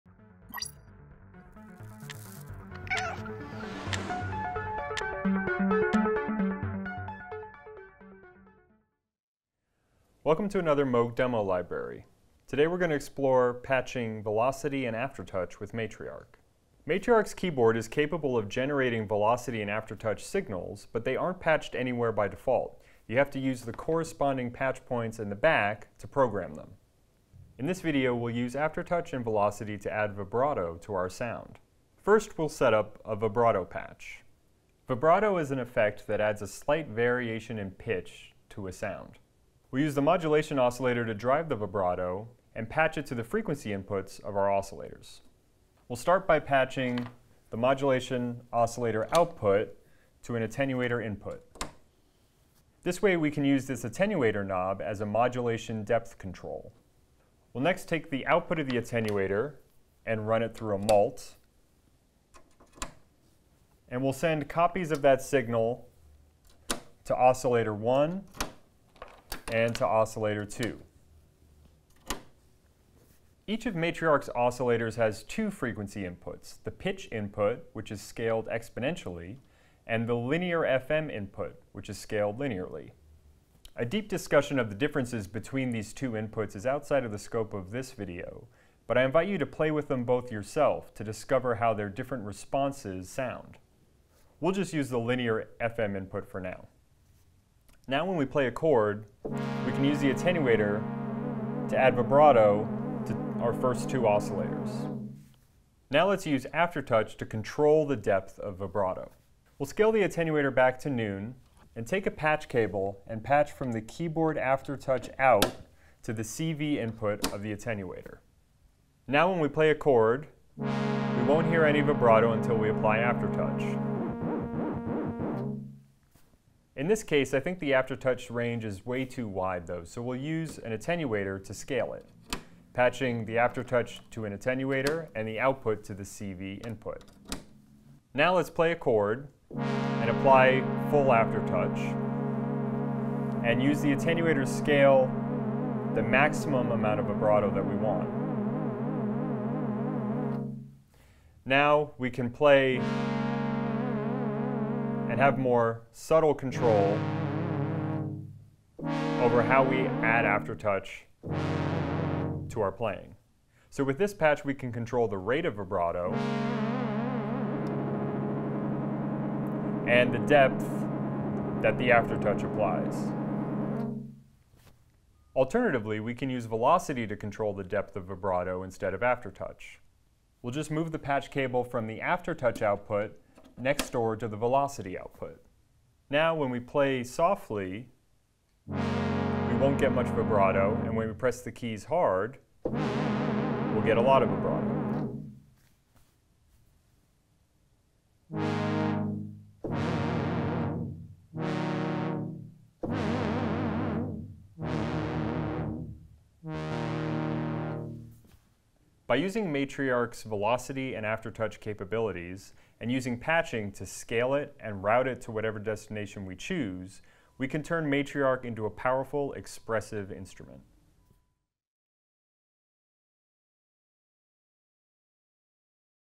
Matriarch | Patching Velocity & Aftertouch
In this video, we walk through how to patch keyboard velocity and aftertouch with Matriarch.

We begin by patching a simple vibrato patch, sending the modulation oscillator of Matriarch to the first two oscillators in its oscillator bank and using an attenuator to scale the depth of vibrato. While this is already possible using Matriarch’s Modulation section and mod wheel, creating the patch this way allows us to add dynamic control of the modulation depth. Next, we show how to dynamically control the vibrato depth with the Matriarch keyboard—first with its aftertouch CV output, and then with its velocity CV output.